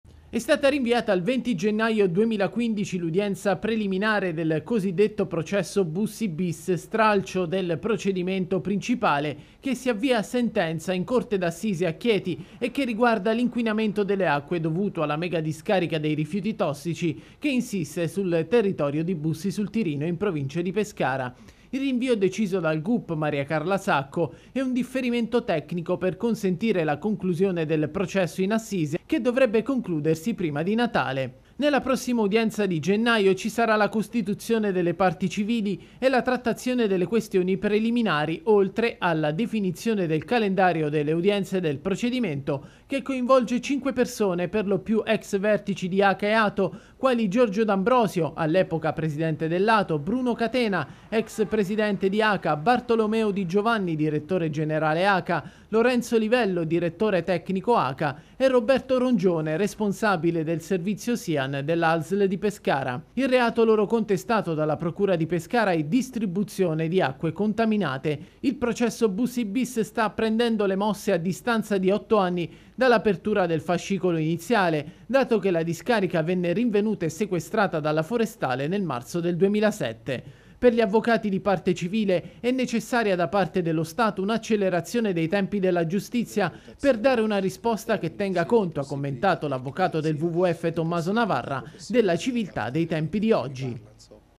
Bussi Bis. Rinviata l’udienza preliminare
Rinvio tecnico al 20 gennaio per l’udienza preliminare del Procedimento Bussi Bis che riguarda la discarica dei veleni e per il quale sono indagate cinque persone per distribuzione di acque contaminate